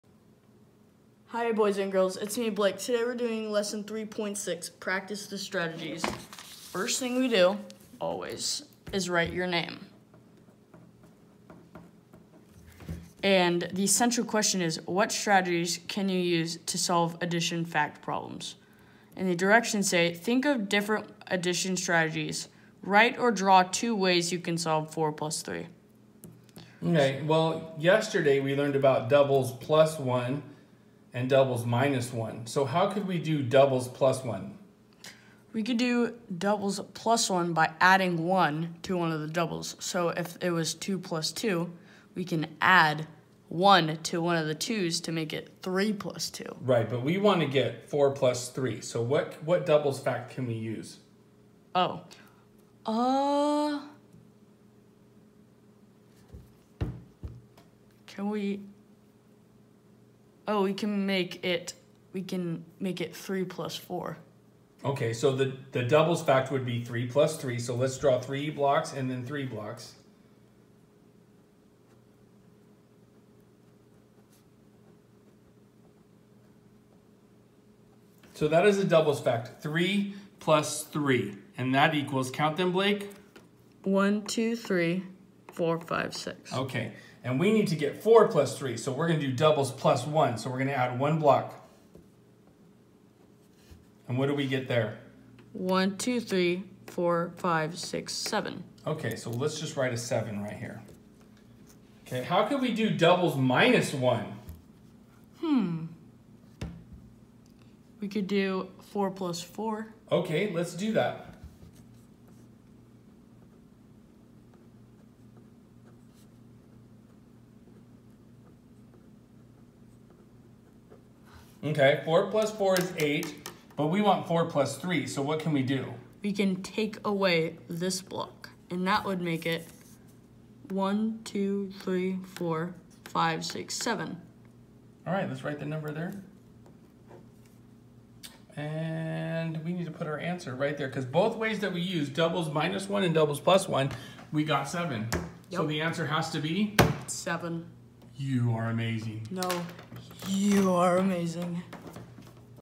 Go math Chapter 3 lesson six or lesson 3.6 first grade for kids
Blake demonstrates how to do the first page of lesson 3.6 first grade for kids